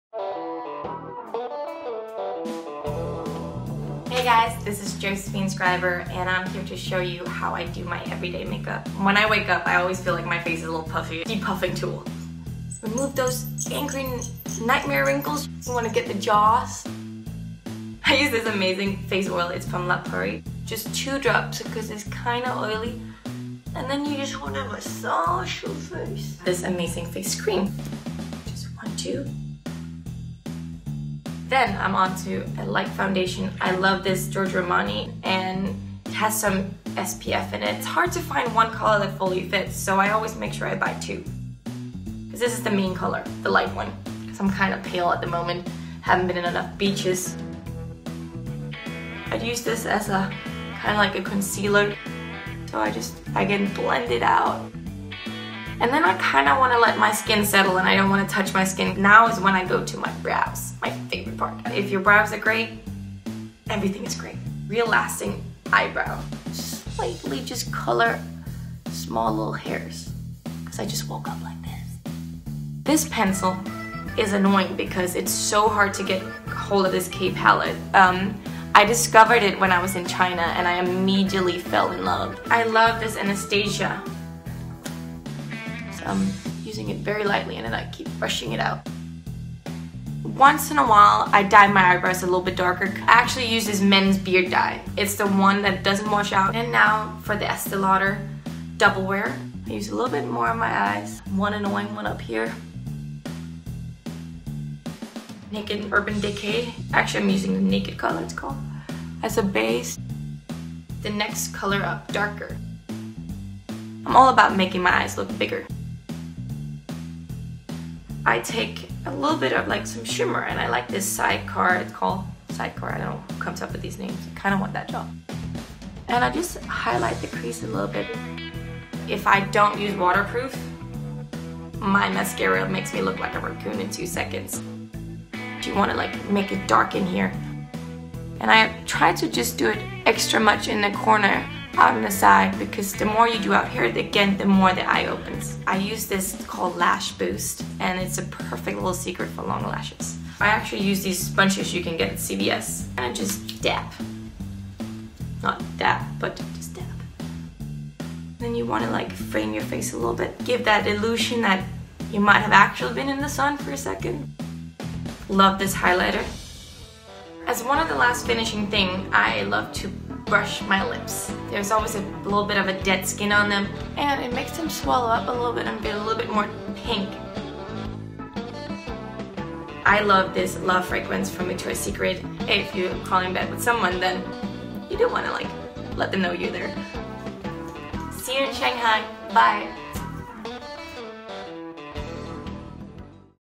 Victoria’s Secret Angel Josephine Skriver's Lip-Plumping Secret | Beauty Secrets | Vogue
For Victoria’s Secret Angel Josephine Skriver, being runway ready for the company’s Shanghai show means serious brow maintenance . . . and one foolproof insider trick for fuller lips.

Danish model Josephine Skriver is a master of the bombshell skin reveal—from her year-round beach-ready abs to her mile-long legs—in the name of work, play, and the Raiders. But an inside look at the Victoria’s Secret model and die-hard football fan’s morning beauty routine, replete with a “de-puffing” face wand, brow dye, and a stealth lip-plumping tool, is a rarity.
With her golden lengths tied into a high ponytail, Skriver—who is already prepping for the iconic lingerie brand’s highly anticipated Shanghai show later this month—announces that she typically wakes up with a puffy visage. (Angels—they’re just like us!) To remove those “angry nightmare wrinkles,” as she calls them, she uses a gemstone roller along her forehead, cheekbones, and jaw. Next, she massages in two drops of face oil by La Prairie, followed by two shades of SPF-infused liquid foundation by Giorgio Armani that she blends by hand to get the perfect skin tone match...

Filmed by Lucas Flores Piran
Filmed at the Beekman, a Thompson Hotel